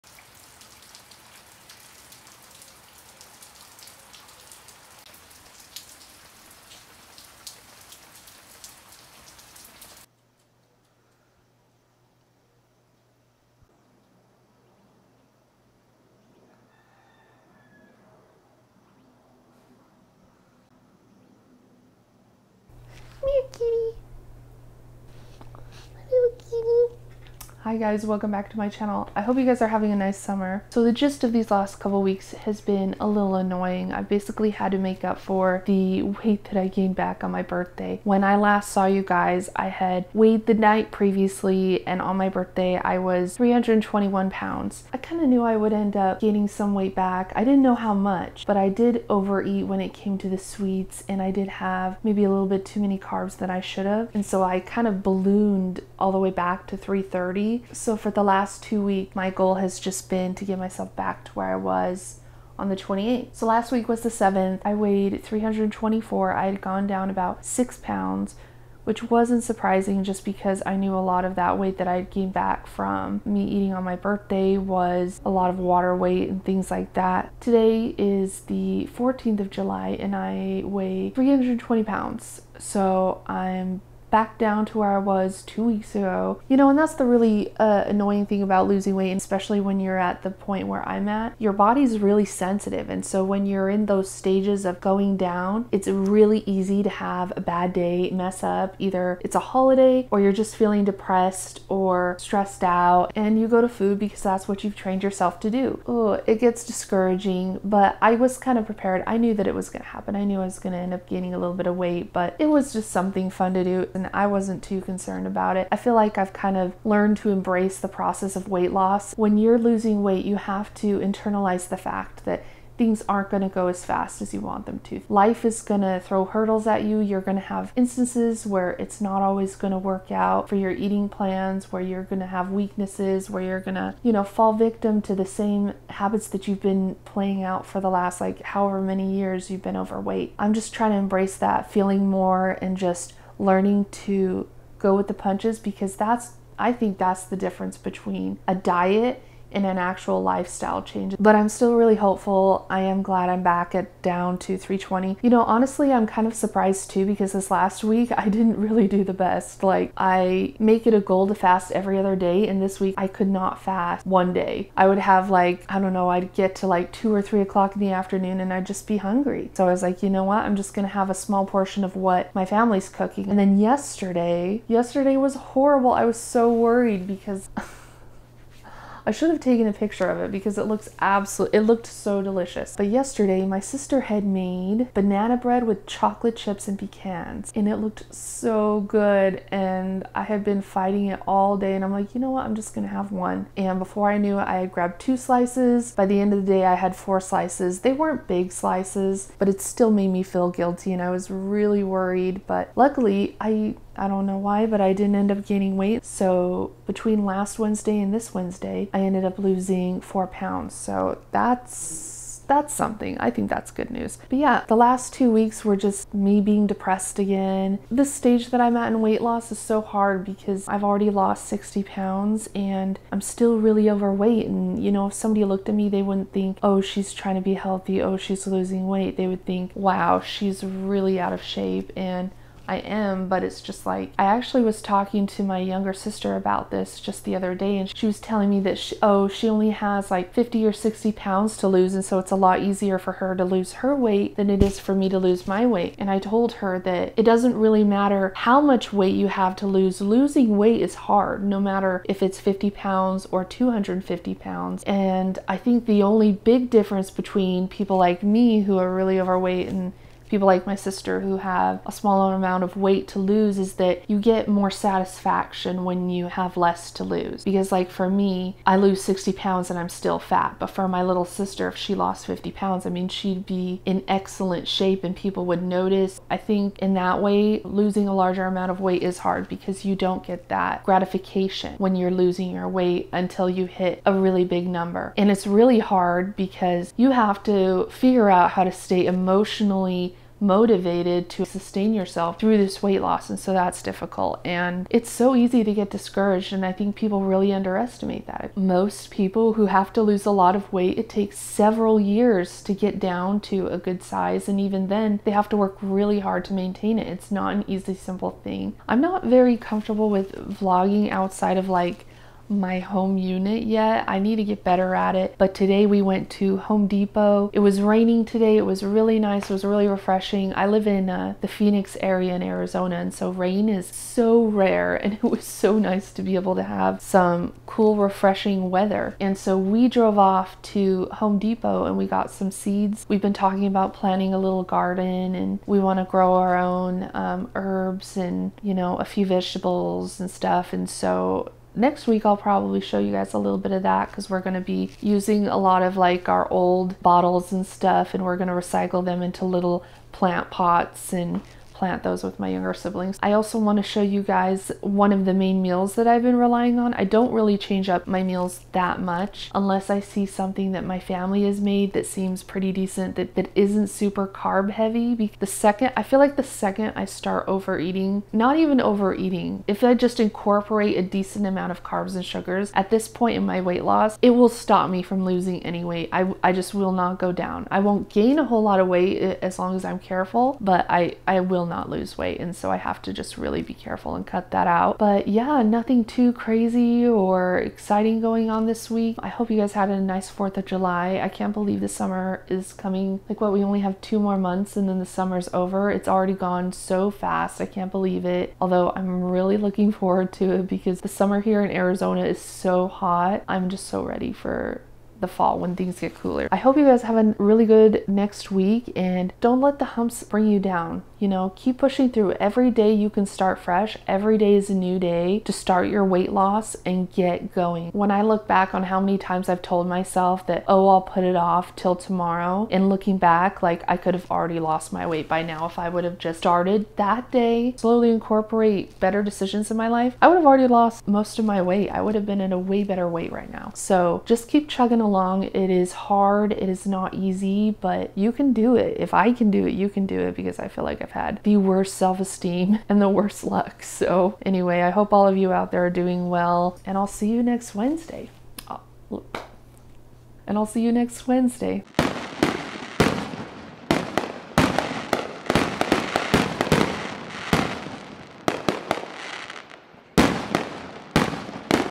MAKING UP FOR WEIGHT LOSS MISTAKES | Weight Loss Journey! 💖 🏋️ Vlog 4
So I ended up gaining back some weight during my birthday on the 28th of last month, so the last two weeks I've spent getting back to where I was. I am now officially back at 320 and ready to go down even further these next few weeks. Don't let a few bad days keep you from accomplishing your goals. You just have to look at each day as a new opportunity. Happy summer!